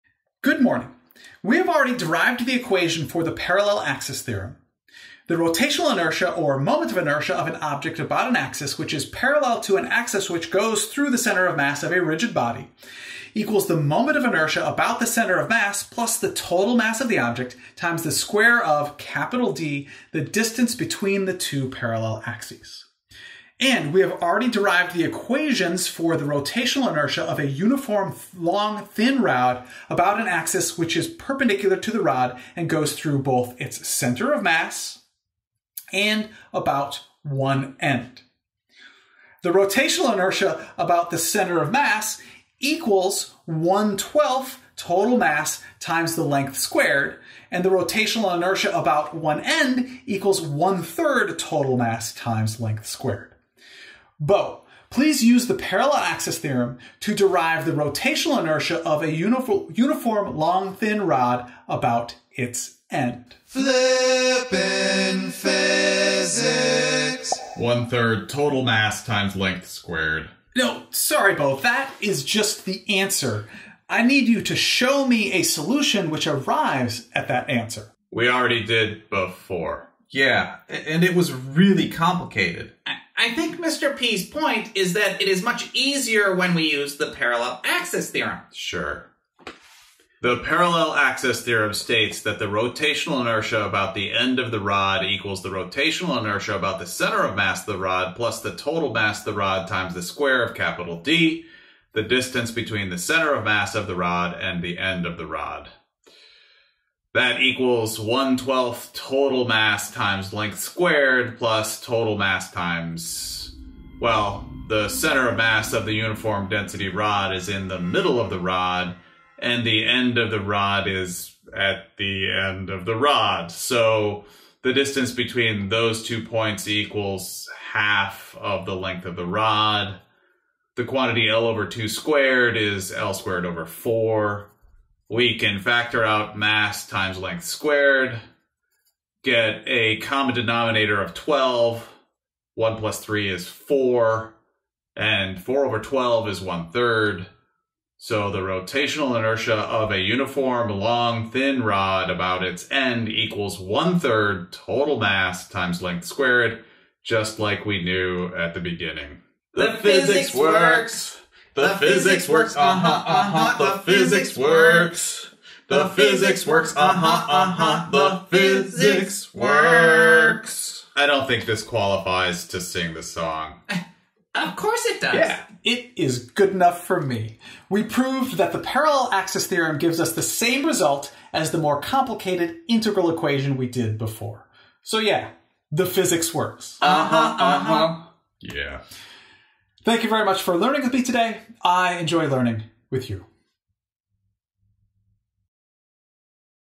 Parallel Axis Theorem Example
Thin Rod example of the Parallel Axis Theorem. This is an AP Physics C: Mechanics topic.

Next Video: 2 Masses on a Pulley - Torque Demonstration

Thank you to Julie Langenbruner and Scott Carter being my Quality Control Team for this video.

Content Times:
0:00 The Parallel Axis Theorem
1:29 Using the Parallel Axis Theorem